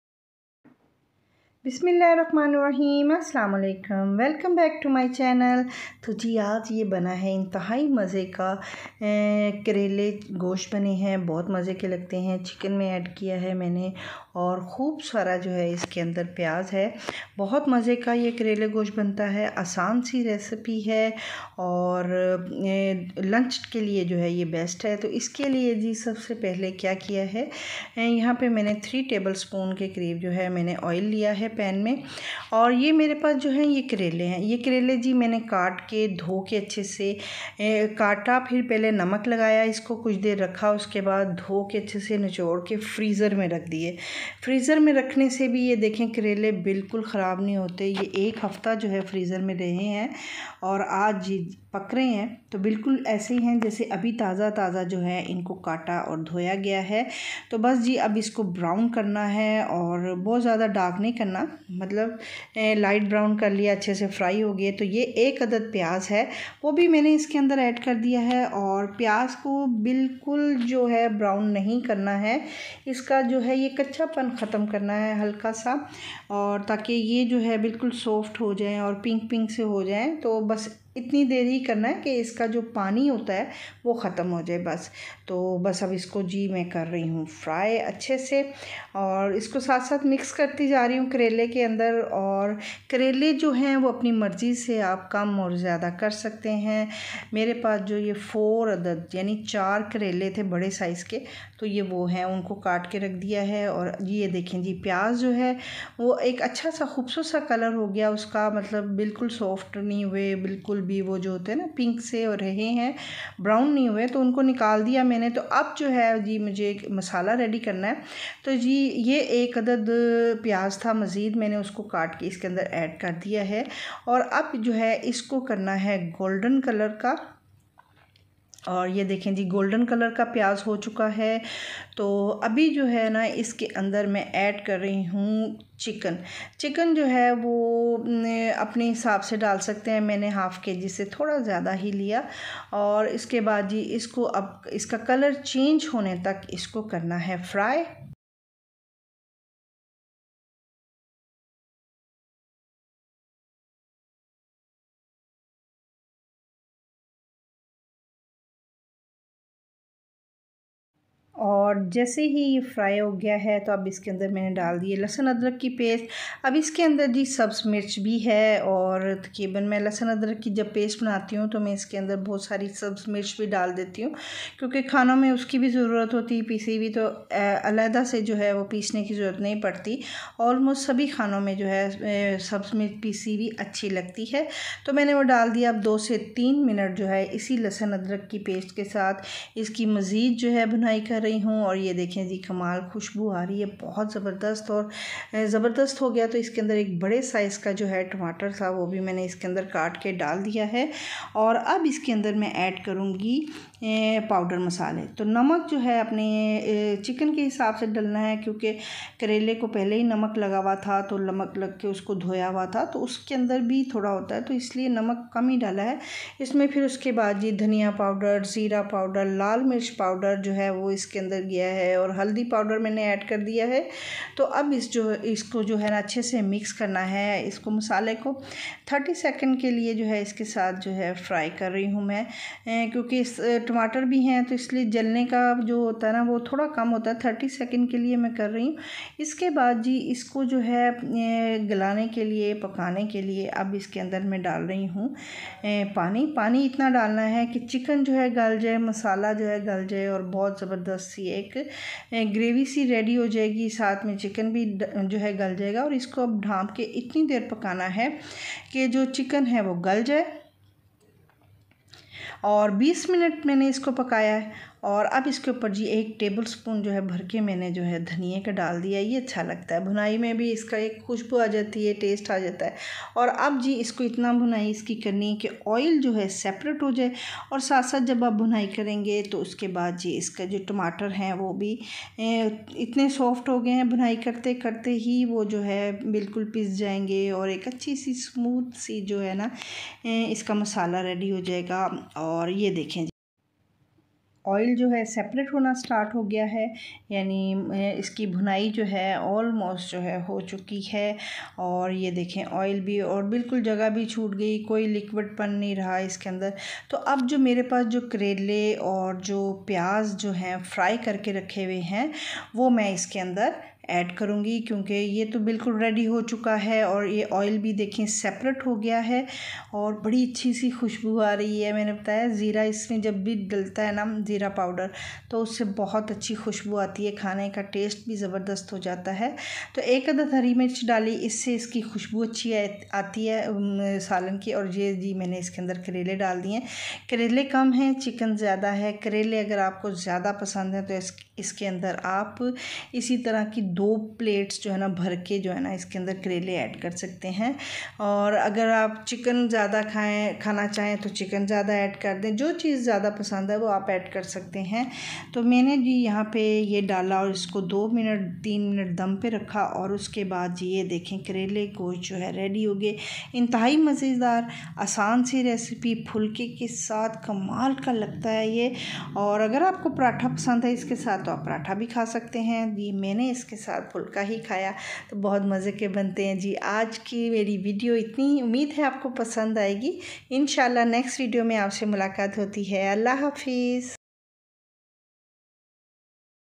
Karelay Gosht Recipe || How To Make Bitter Gourd With Chicken At Home
Karelay chicken asan si recipe h bht mzay k bantay hain umeed h apko Pasand ae gi video